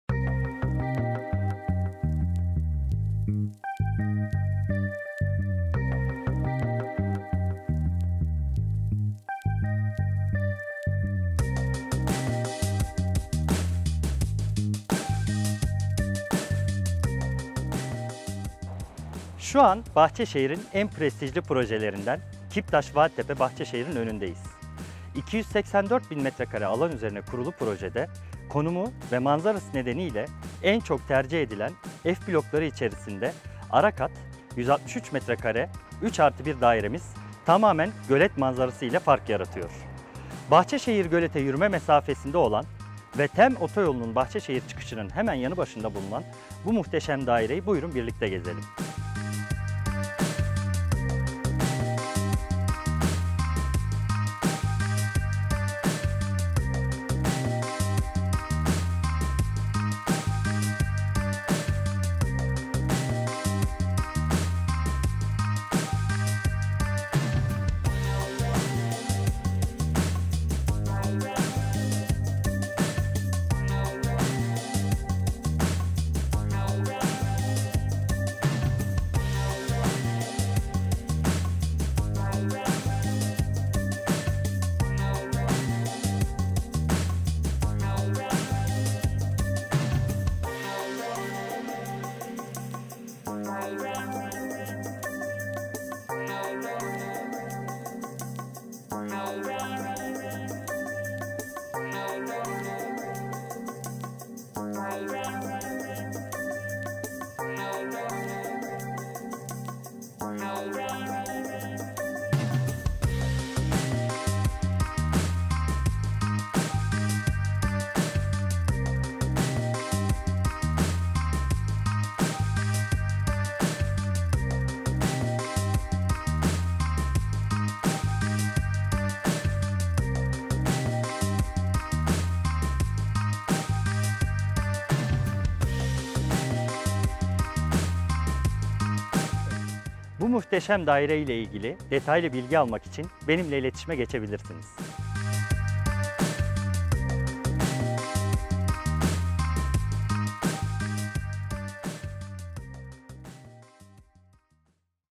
Bahçeşehir Vaditepe evleri satılık daire
Bahçeşehir Vaditepe projesinde satılık daire
Kiptaş Vaditepe evleri Bahçeşehir
Gölet manzaralı
Bahçeşehir şehir manzaralı
Muhteşem bir daire

Detaylar için bizimle iletişime geçebilirsiniz.

yeninesilgayrimenkuldanışmanlığı